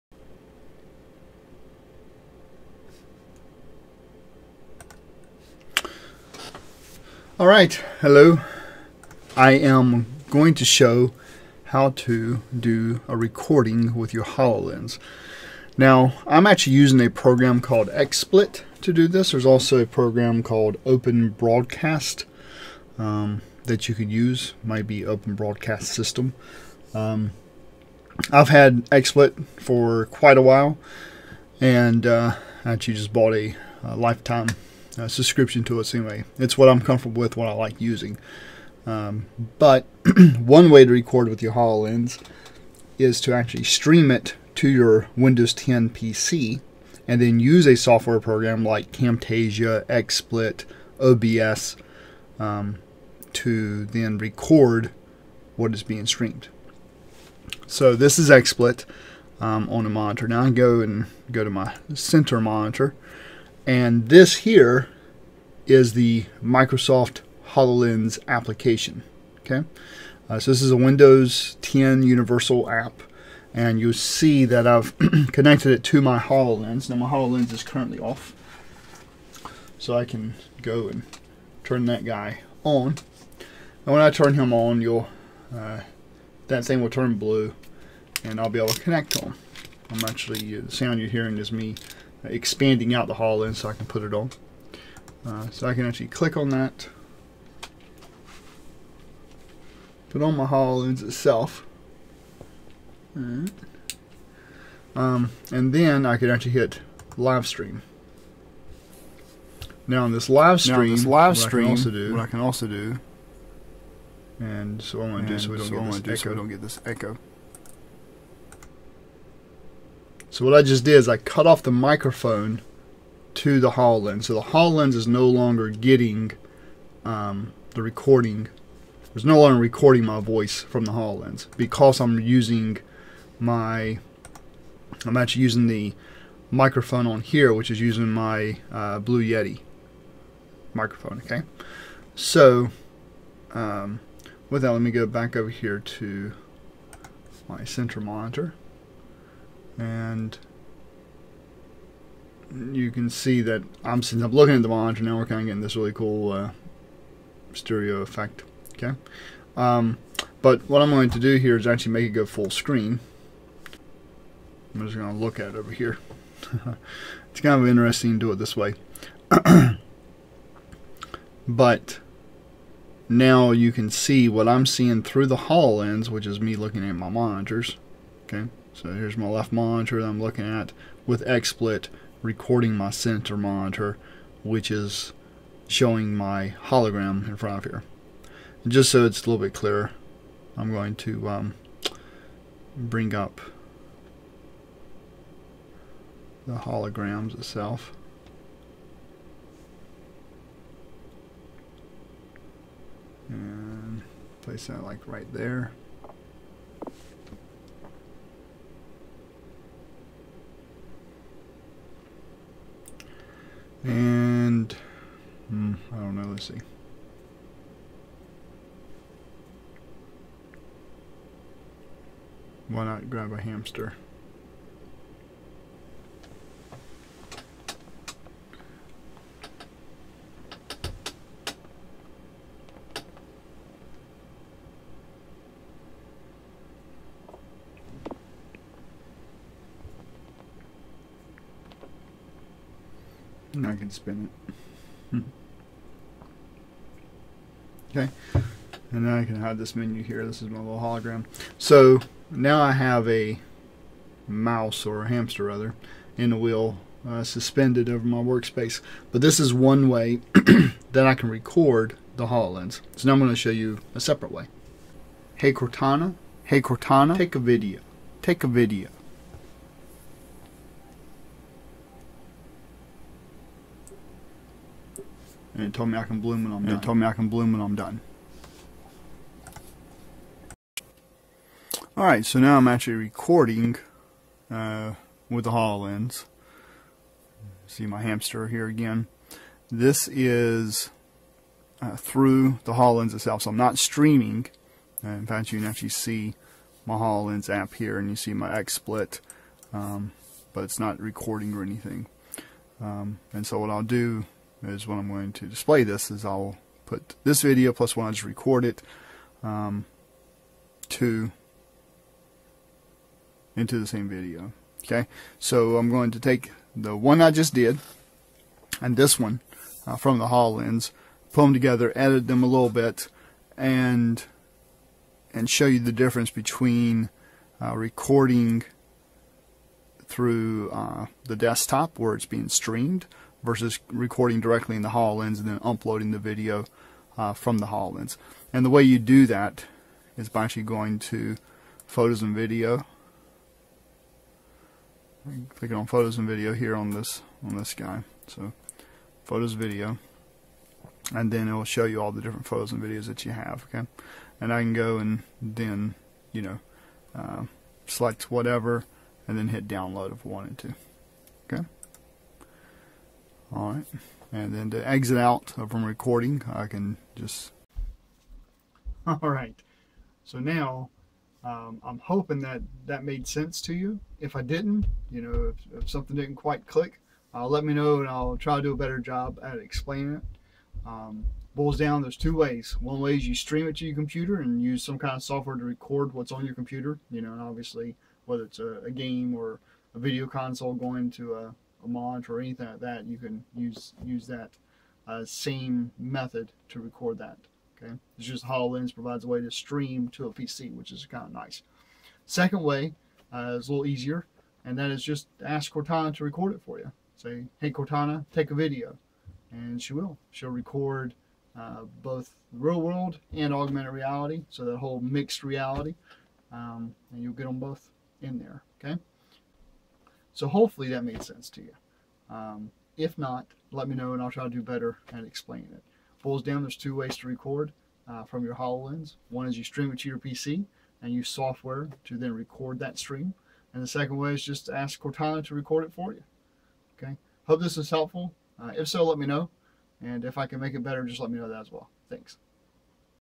How to Record using your HoloLens TWO different ways
Learn two different ways to record using your HoloLens!
1. Stream the HoloLens to your PC and use a screen capturing software to record that stream.
2. Say, "Hey Cortana, Take a Video". She will. You can then download the video to your PC or share it to Facebook or YouTube.